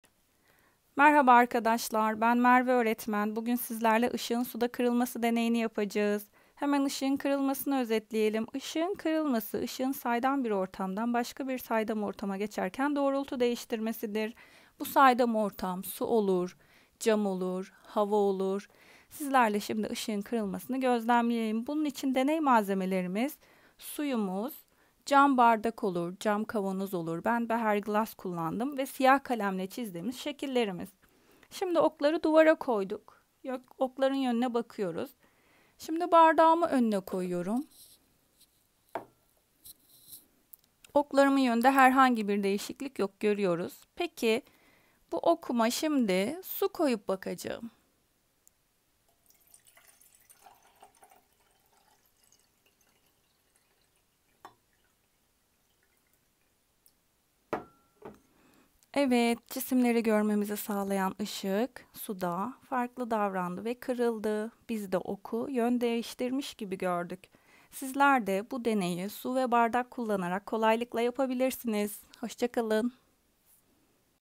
IŞIĞIN KIRILMASI
Işığın, yoğunlukları farklı bir saydam ortamdan başka bir saydam ortama geçerken doğrultu değiştirmesine ışığın kırılması denir. Bardağın içinde kalemin kırık görünmesi, serap olayı ışığın kırılmasına örneklerdir. Işığın kırılmasından dolayı cisimler olduğundan farklı görünebilirler.
Deney malzemeleri: A4 kağıdı, keçeli kalem, beherglas 250 ml ve su
Deneyin Yapılışı: A4 kağıda keçeli kalem ile çizimler yapılır. Su ile doldurulan beherglas çizimlerin önüne konularak gözlem yapılır.